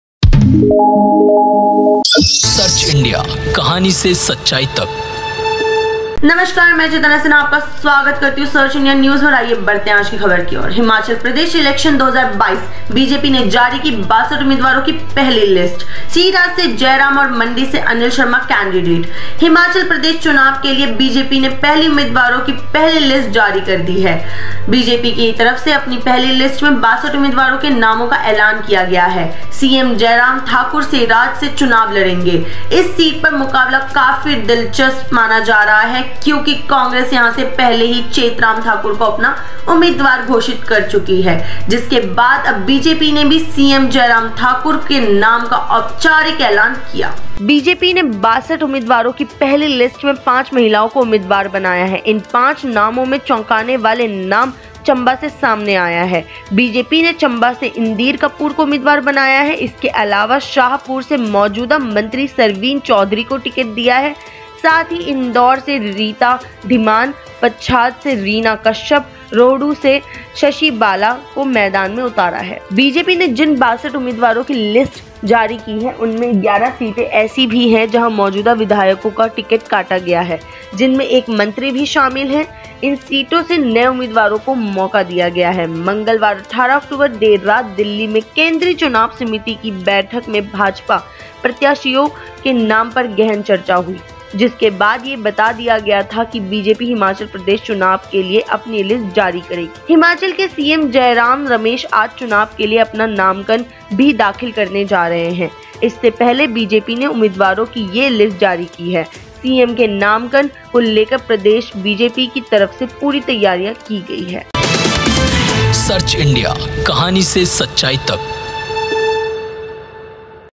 बीजेपी ने अपने उम्मीदवारों की पहली लिस्ट जारी की...
बीजेपी की तरफ से अपनी पहली लिस्ट में 62 उम्मीदवारों के नामों का एलान किया गया है l